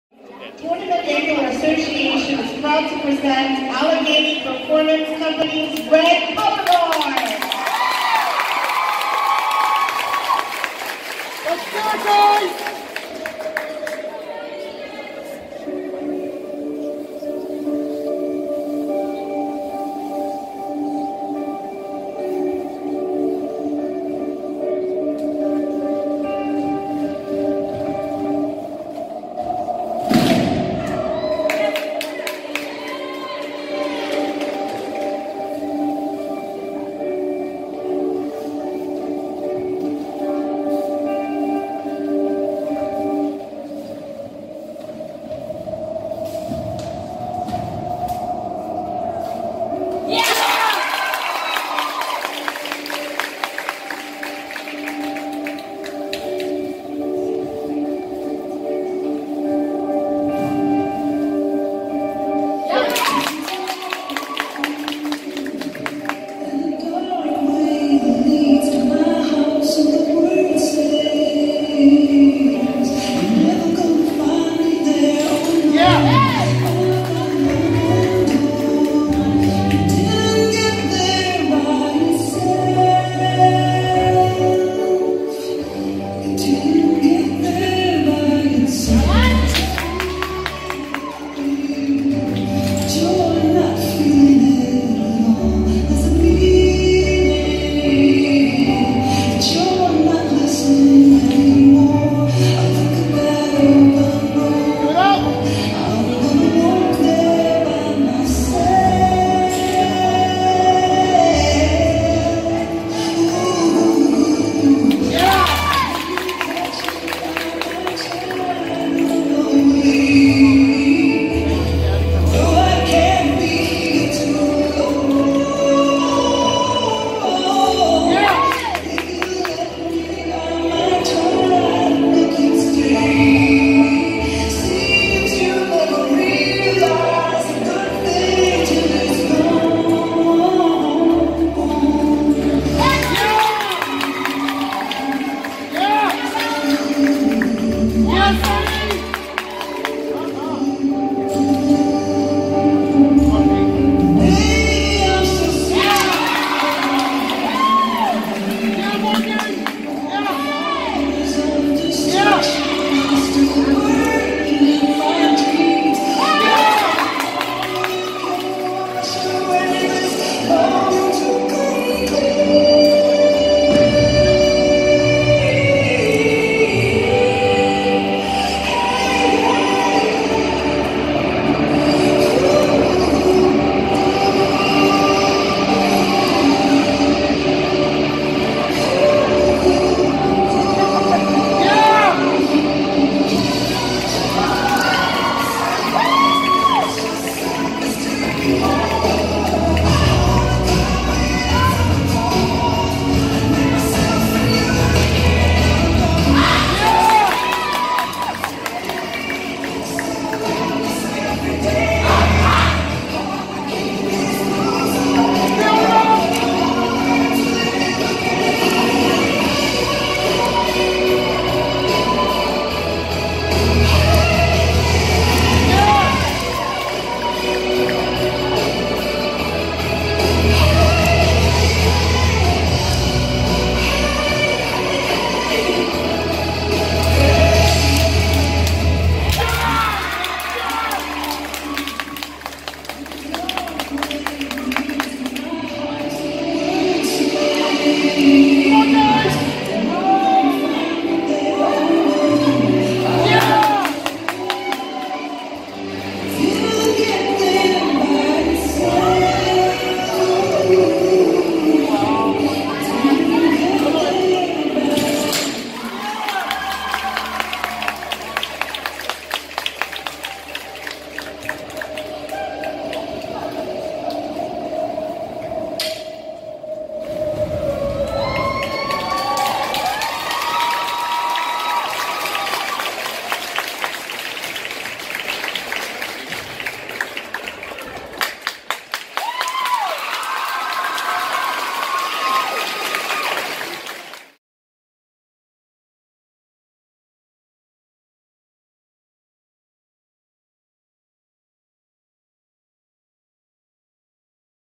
APC Red Guard, Dark Road, Chapter 5 Championship - 4/28/2018
The APC Red Colorguard (Scholastic A Class) performs their 2018 show, Dark Road, during their TIA home show held at Fort Hill High School.